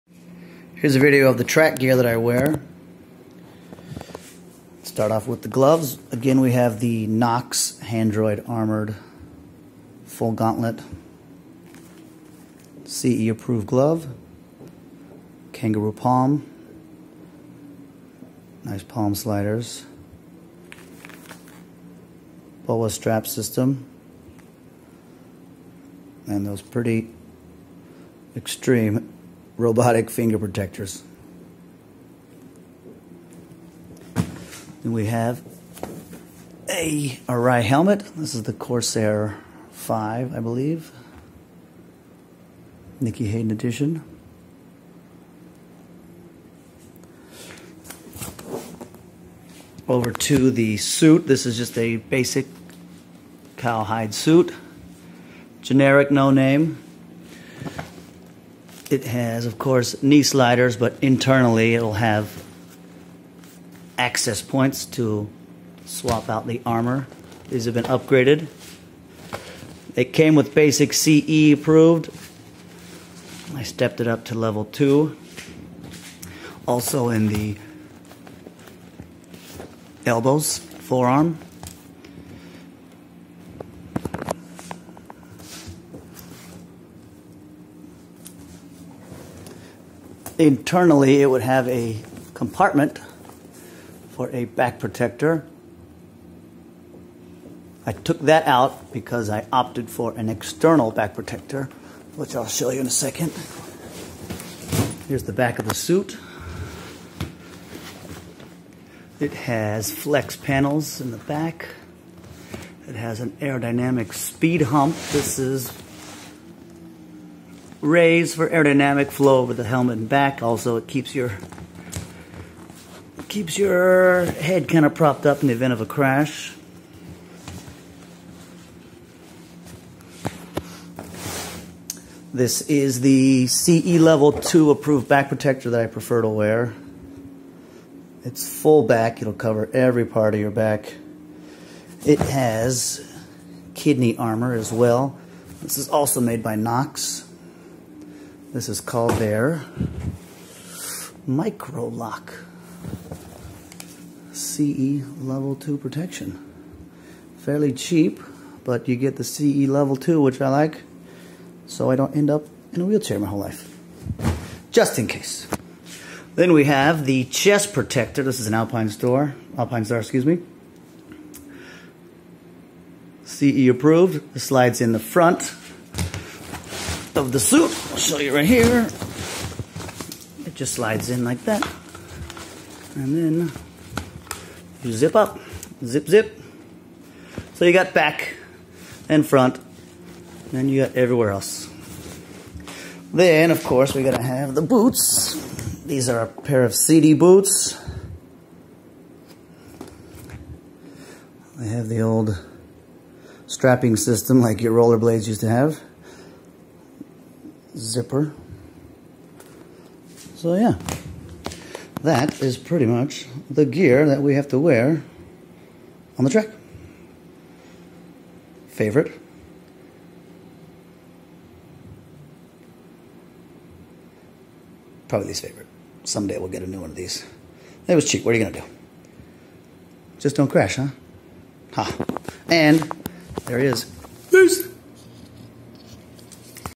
Track Gear Motorcycle Racing Suit, Helmet, Boots, Gloves & Armor 王口一
A look at the gear I wear at the track.

Leather racing suit, all cow leather, CE armor in the shoulders, hip, knees and forearms.

Arai Corsair V Nicky Hayden helmet.

Knox Handroid full gauntlet gloves.

Knox Microlock CE Level 2 back protector.

Alpinestars Nucleon KR-CiR CE Level 2 chest protector.

Sidi ST boots.